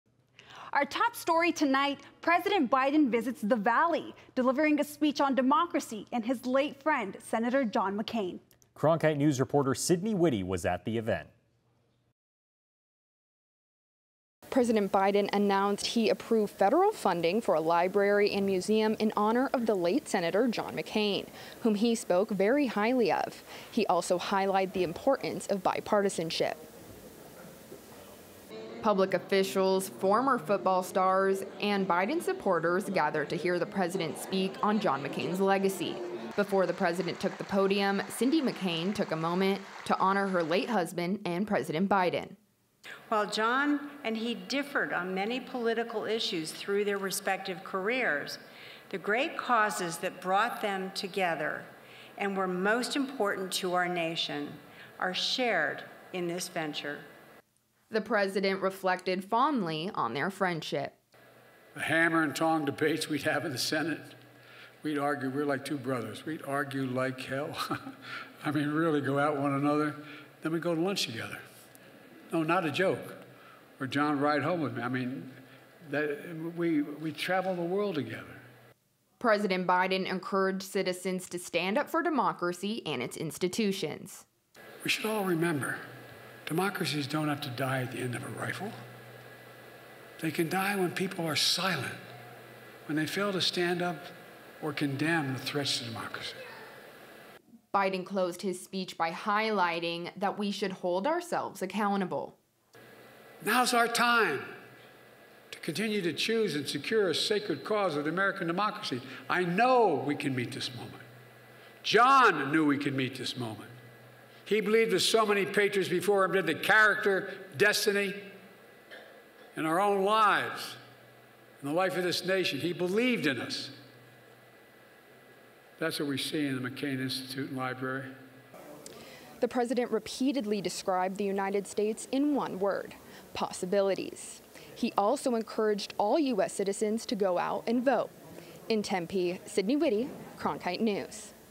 President Biden Visit | Cronkite News
President Joe Biden is in Phoenix Thursday where he announced the creation of a John McCain Memorial Library at ASU and attended a number of fundraising events. (Video by Sydney Witte/Cronkite News)